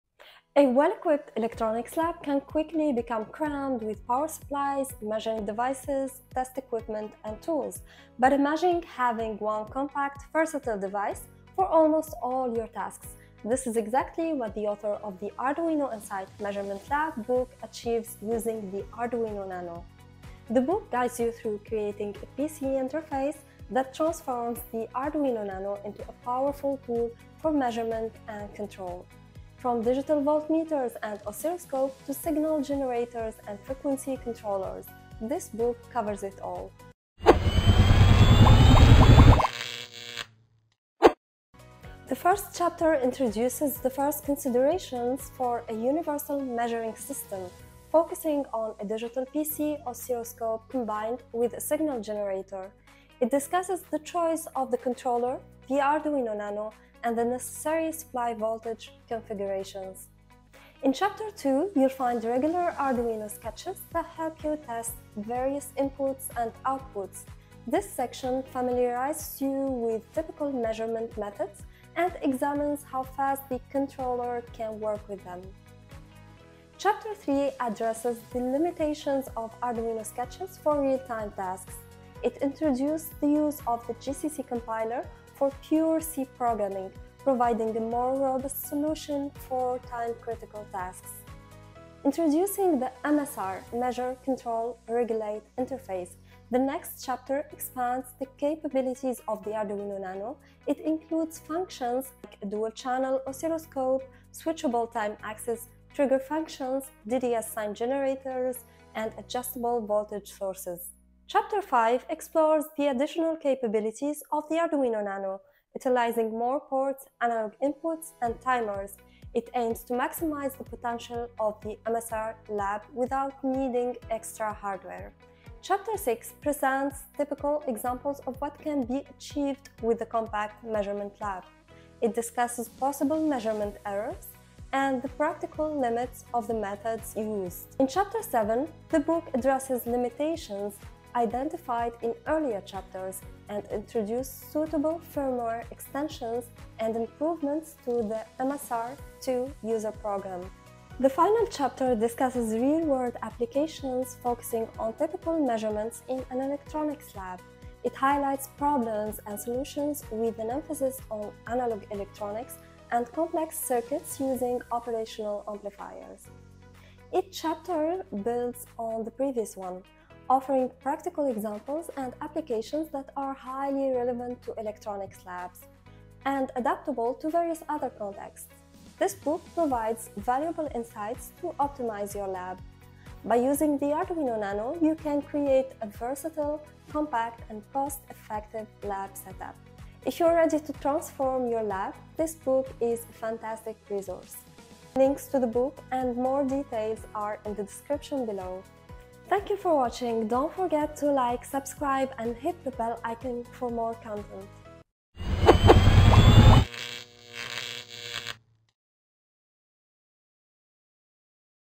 The Arduino-Inside Measurement Lab
A well-equipped electronics lab is crammed with power supplies, measuring devices, test equipment and signal generators.

Wouldn‘t it be better to have one compact device for almost all tasks? Based on the arduino a PC interface is to be developed that’s as versatile as possible for measurement and control
It simply hangs on a USB cable and – depending on the software – forms the measuring head of a digital voltmeter or PC oscilloscope, a signal generator, an adjustable voltage source, a frequency counter, an ohmmeter, a capacitance meter, a characteristic curve recorder, and much more.

The circuits and methods collected here are not only relevant for exactly these tasks in the "MSR" electronics lab, but many details can also be used within completely different contexts.